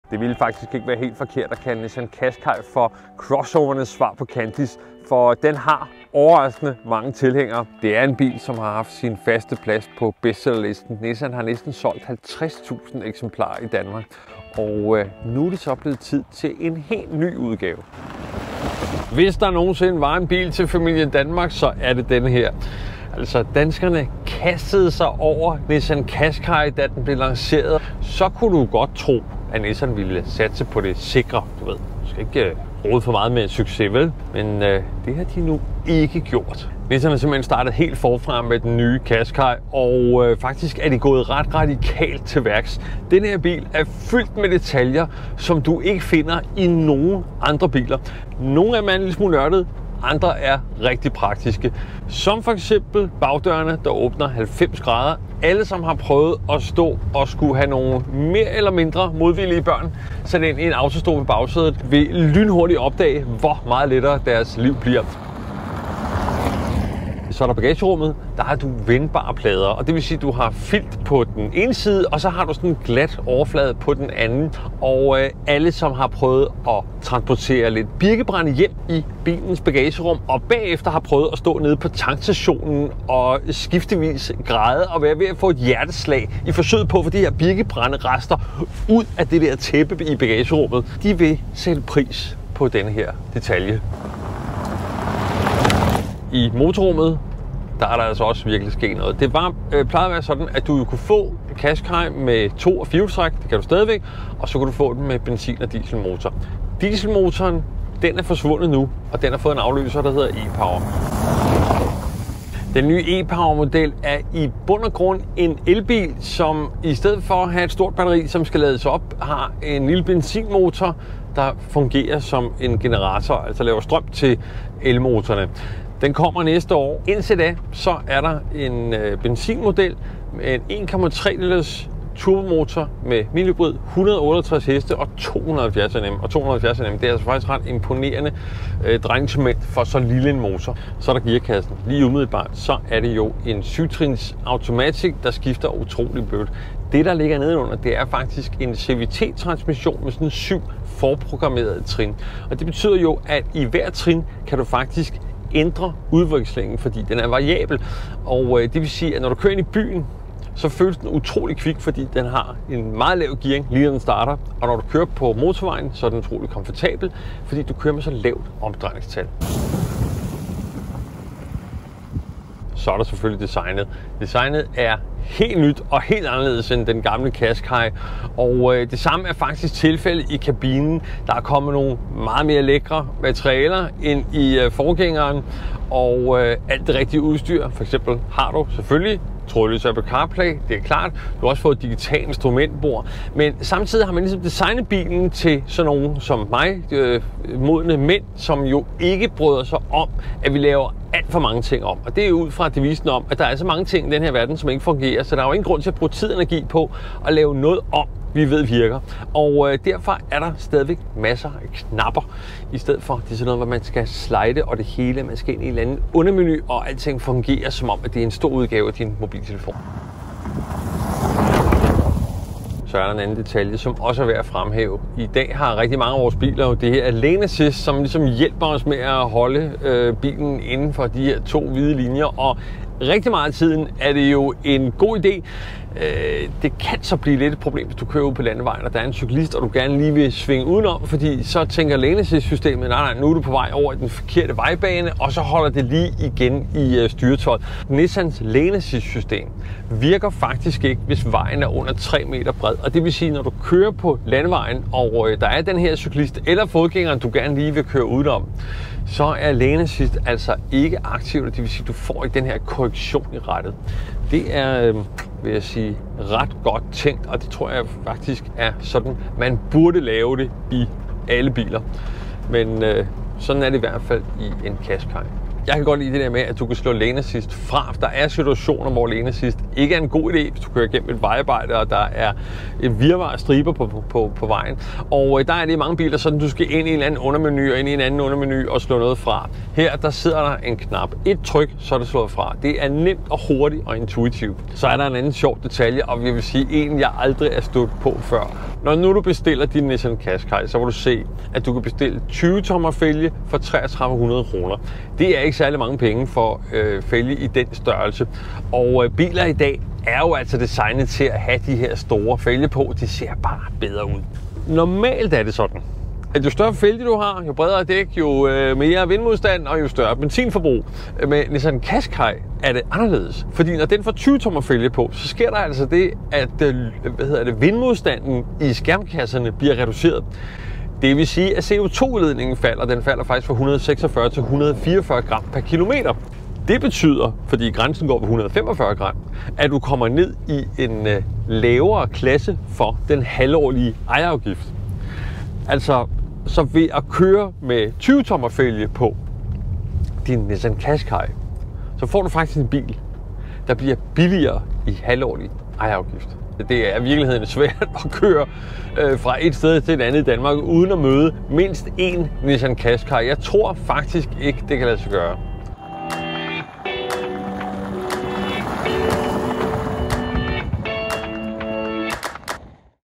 [2021] Ny Nissan Qashqai
Hvis der er en bil til familien Danmark, så er den her! Nissan har solgt næsten 50.000 eksemplarer af den første Qashqai herhjemme, og nu er det så tid til en helt ny model. Den har bagdøre som åbner 90 grader, kommer i en elektrisk udgave som ikke skal oplades og så har den naturligvis et interiør, skabt til modne mænd af den slags, som ikke mener, at alting skal være som din mobiltelefon, men gerne vil have trådløs Apple CarPlay som standard!